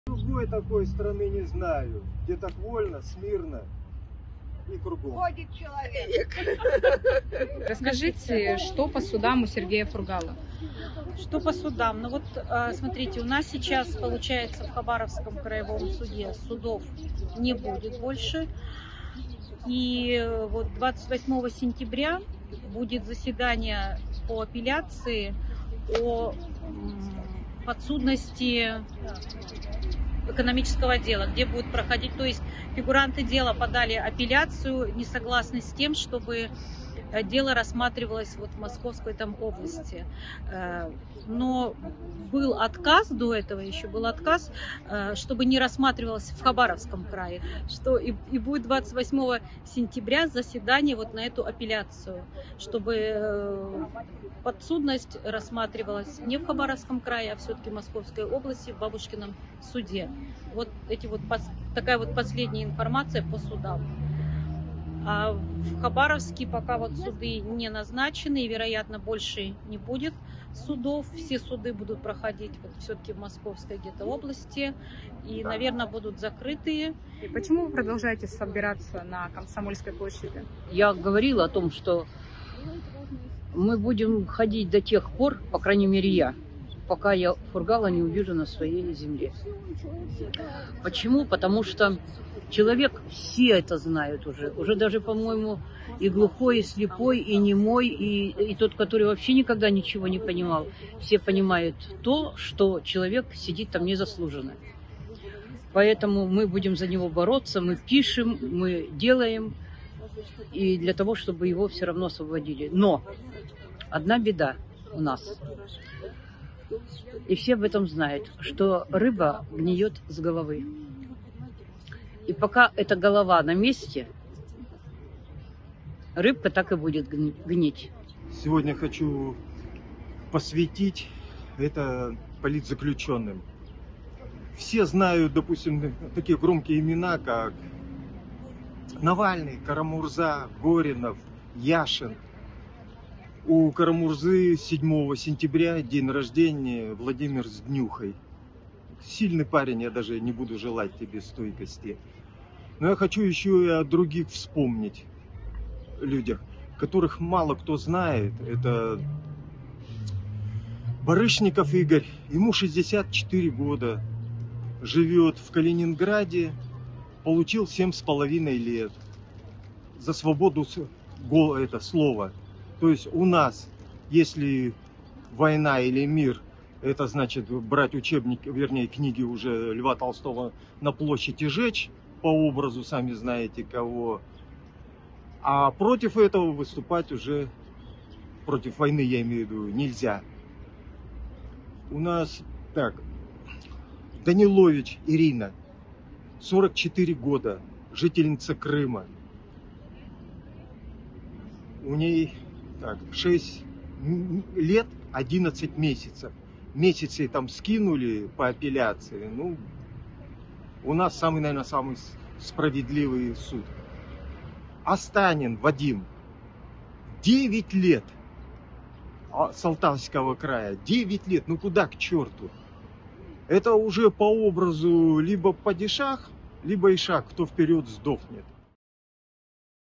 🟠«Рыба гниёт с головы» | 821-й день Бессрочного пикета
«Рыба гниёт с головы»

821-й день Бессрочного пикета в Хабаровске в защиту Сергея Фургала и всех политзаключённых.